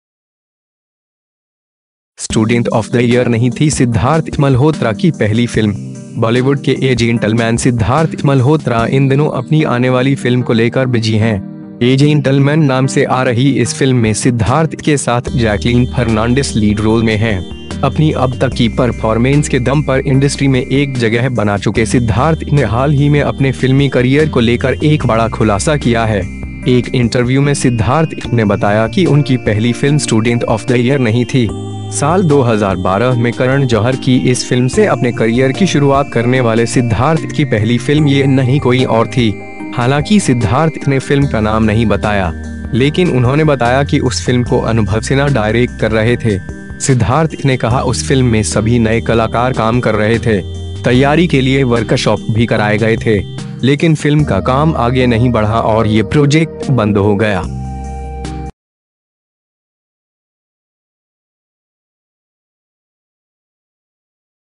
'स्टूडेंट ऑफ द ईयर' नहीं थी सिद्धार्थ मल्होत्रा की पहली फिल्म!
बॉलीवुड के 'ए जेंटलमैन' सिद्धार्थ मल्होत्रा इन दिनों अपनी आने वाली फिल्म को लेकर बिजी हैं. 'ए जेंटलमैन' नाम से आ रही इस फिल्म में सिद्धार्थ के साथ जैकलीन फर्नांडिस लीड रोल में हैं. अपनी अब तक की परफॉर्मेंस के दम पर इंडस्ट्री में एक जगह बना चुके सिद्धार्थ ने हाल ही में अपने फिल्मी करियर को लेकर एक बड़ा खुलासा किया है.
एक इंटरव्यू में सिद्धार्थ ने बताया कि उनकी पहली फिल्म स्टूडेंट ऑफ द ईयर नहीं थी. साल 2012 में करण जौहर की इस फिल्म से अपने करियर की शुरुआत करने वाले सिद्धार्थ की पहली फिल्म ये नहीं कोई और थी.
हालांकि सिद्धार्थ ने फिल्म का नाम नहीं बताया. लेकिन उन्होंने बताया कि उस फिल्म को अनुभव सिन्हा डायरेक्ट कर रहे थे. सिद्धार्थ ने कहा, उस फिल्म में सभी नए कलाकार काम कर रहे थे. तैयारी के लिए वर्कशॉप भी कराए गए थे. लेकिन फिल्म का काम आगे नहीं बढ़ा और ये प्रोजेक्ट बंद हो गया.